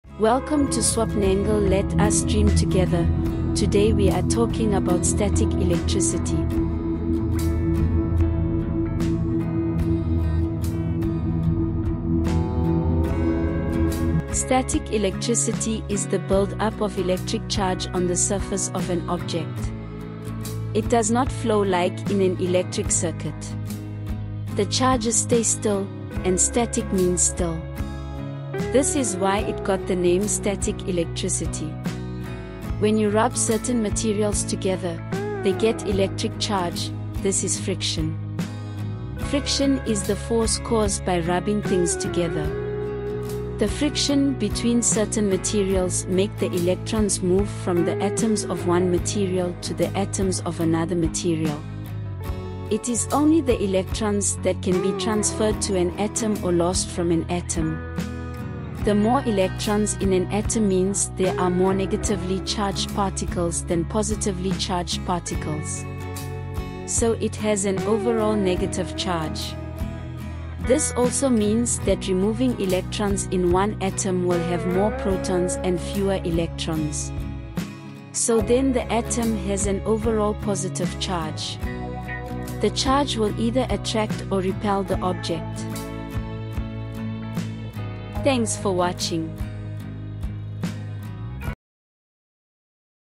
Grade 8 - Static Electricity
Static electricity is the build-up of electric charge on the surface of an object. it does not flow like in an electric circuit. The charges stay still, and static means still. This is why it got the name static electricity.
When you rub certain materials together, they get electric charge, this is friction. Friction is the force caused by rubbing things together. The friction between certain materials make the electrons move from the atoms of one material to the atoms of another material. It is only the electrons that can be transferred to an atom or lost from an atom.
The more electrons in an atom means there are more-negatively charged particles than positively charged particles. So it has an overall negative charge. This also means that removing electrons in one atom will have more protons and fewer electrons. So then the atom has an overall positive charge. The charge will either attract or repel the object.